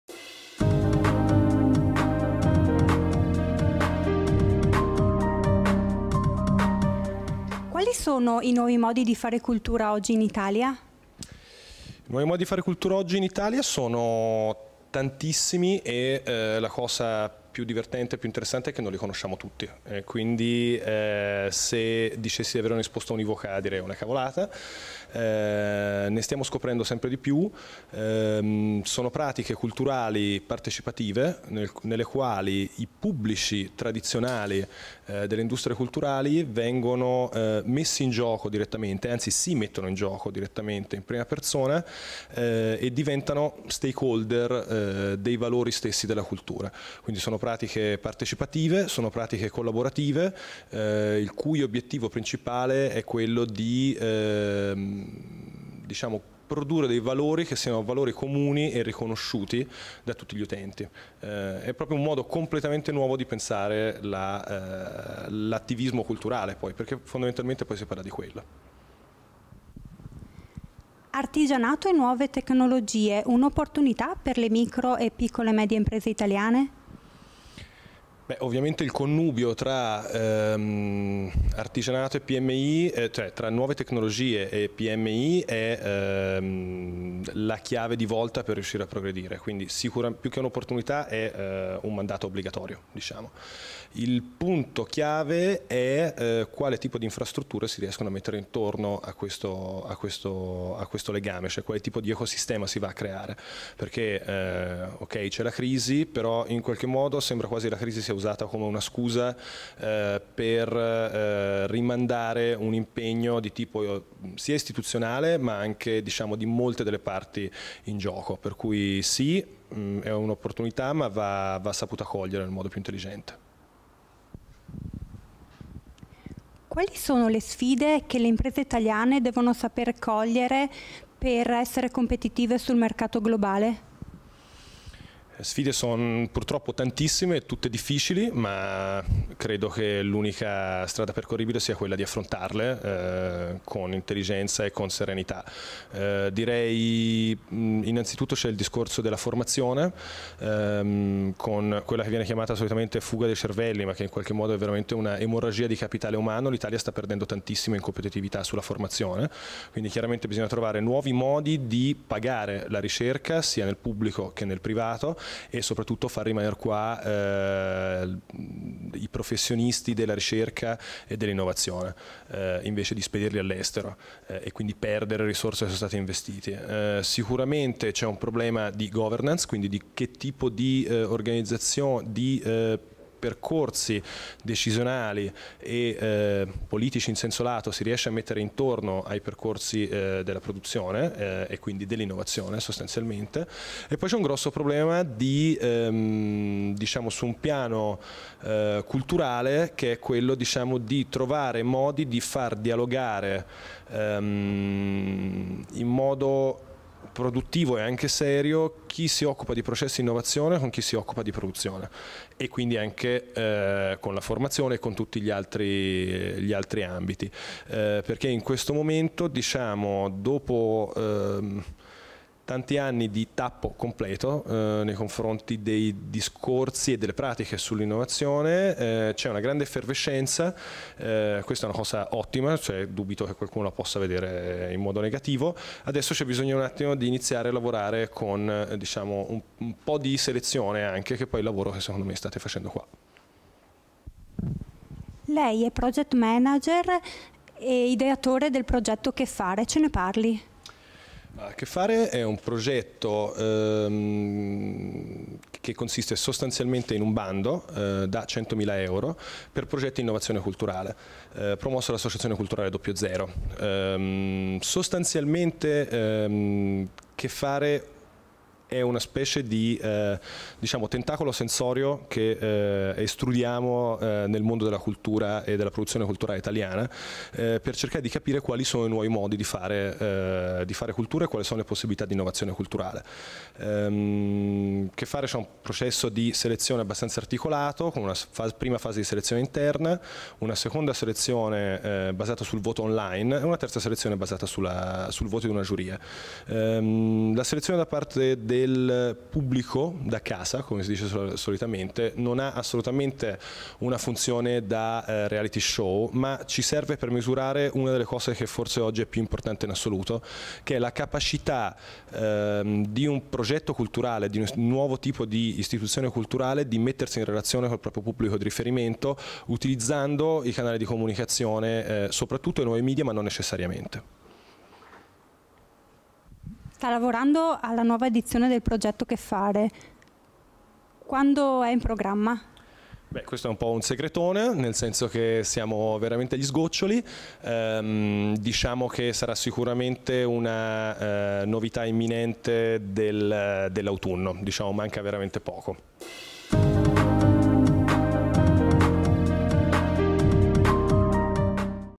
Future Forum 2013 - Bertram Niessen: Manifatture digitali e trasformazioni socio-culturali
- Al Future Forum abbiamo incontrato Bertram Niessen professore alla NABA di Milano e project manager del Progetto cheFare. 23-10-13, Udine, BCC Manzano Filiale di Udine.